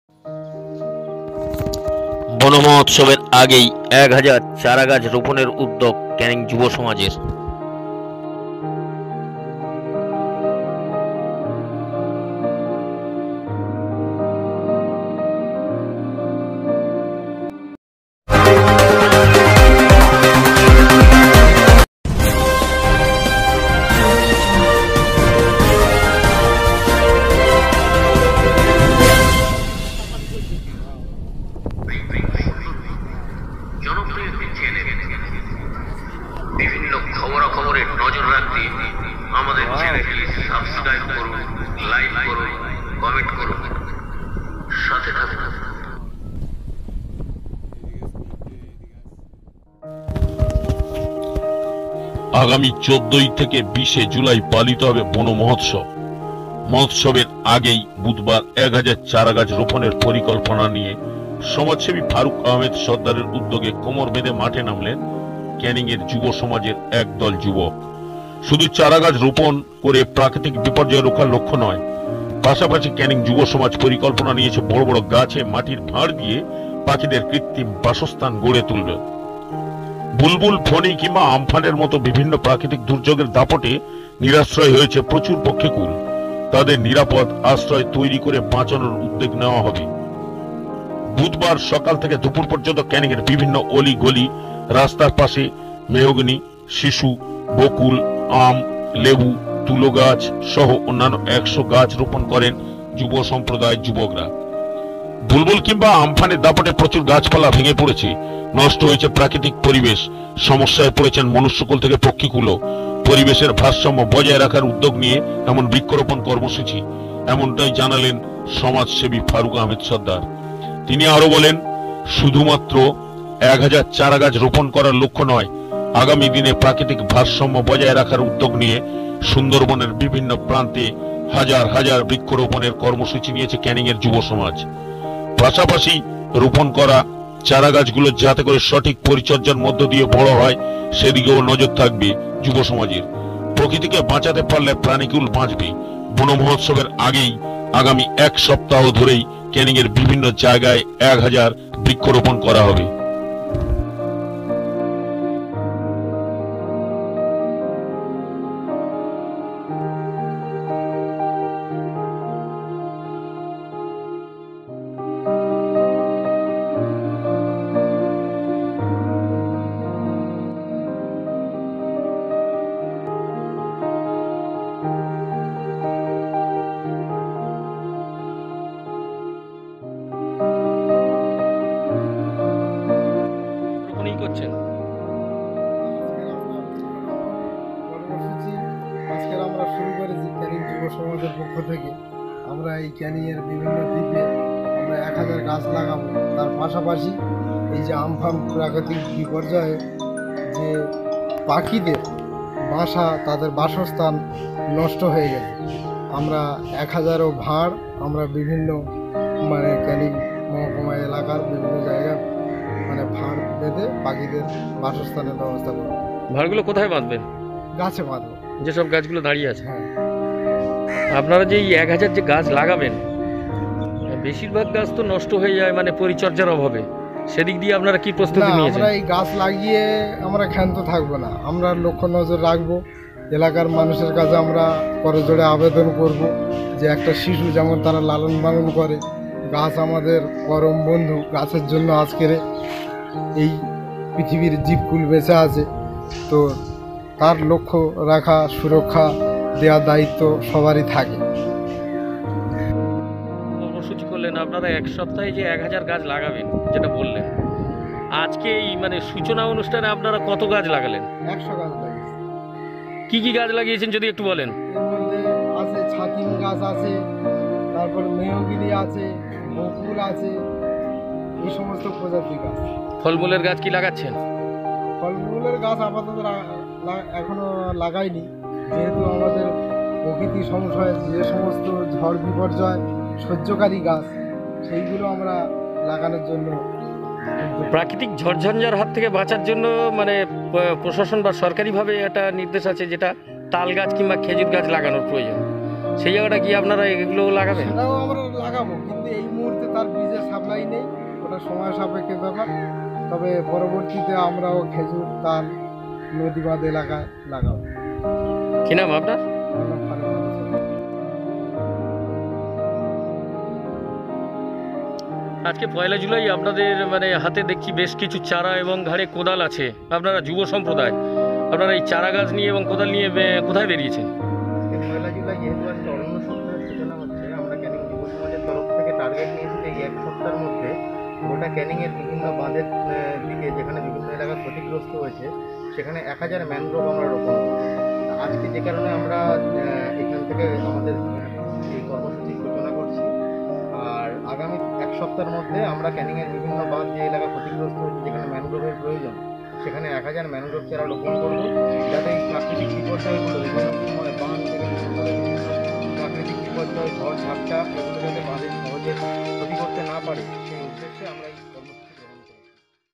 বনমোহৎসবের আগেই এক হাজার চারাগাছ রোপণের উদ্যোগ ক্যানিং যুবসমাজের
সুভাষ চন্দ্র দাশ,ক্যানিং -আগামী ১৪-২০ জুলাই পালিত হবে বনমোহৎসব।বনমোহৎসব এর আগেই বুধবার এক হাজার চারাগাছ রোপণের পরিকল্পনা নিয়ে সমাজসেবী ফারুক আহমেদ সরদারের  উদ্যোগে কোমর বেঁধে মাঠে নামলেন ক্যানিং যুবসমাজের একদল যুবক।শুধু চারাগাছ রোপণ করে প্রাকৃতিক  বিপর্যয় রোখার লক্ষ্য নয়।পাশাপাশি ক্যানিং যুবসমাজ পরিকল্পনা নিয়েছে বড় বড় গাছে মাটীর ভাঁড় দিয়ে পাখিদের কৃত্রিম বাসস্থান গড়ে তোলা হবে।বুলবুল,ফণী কিংবা আম্ফানের মতো বিভিন্ন প্রাকৃতিক দুর্যোগের দাপটে নিরাশ্রয় হয়েছে প্রচুর পক্ষীকুল।তাদের নিরাপদ আশ্রয় তৈরী করে বাঁচানোর উদ্যোগ নেওয়া হবে।বুধবার সকাল থেকে দুপুর পর্যন্ত ক্যানিংয়ের বিভিন্ন অলিগলি রাস্তার পাশে মেহগণি,শিশু,বকুল,আম,লেবু,তুলো গাছ সহ অন্যান্য ১০০ গাছ রোপণ করেন যুবসম্প্রদায়ের যুবকরা।
উল্লেখ্য বুলবুল কিংবা আম্ফানের দাপটে প্রচুর গাছপালা ভেঙে পড়েছে। নষ্ট হচ্ছে প্রাকৃতিক পরিবেশ।সমস্যা পড়েছেন মনুষ্যকুল থেকে পক্ষীকুলও।পরিবেশের ভারসাম্য বজায় রাখার উদ্যোগ নিয়ে এমন বৃক্ষরোপণ কর্মসুচি । এমনটাই জানালেন সমাজসেবী ফারুক আহমেদ সরদার। তিনি আরো বলেন শুধুমাত্র একহাজার চারাগাছ রোপণ করার লক্ষ্য নয়। আগামী দিনে প্রাকৃতিক ভারসাম্য বজায় রাখার উদ্যোগ নিয়ে সুন্দরবনের বিভিন্ন প্রান্তে হাজার হাজার বৃক্ষরোপণের কর্মসুচী নিয়েছে ক্যানিংয়ের যুবসমাজ।পাশাপাশি রোপণ করা চারা গাছ গুলো যাতে করে সঠিক পরিচর্যার মধ্যদিয়ে বড় হয় সেদিকেও নজর থাকবে যুবসমাজের।প্রকৃতি কে বাঁচাতে পারলে প্রাণীকুল বাঁচবে।বনমোহৎসবের আগেই আগামী এক সপ্তাহ ধরে ক্যানিংয়ের বিভিন্ন জায়গায় এক হাজার বৃক্ষরোপণ করা হবে।”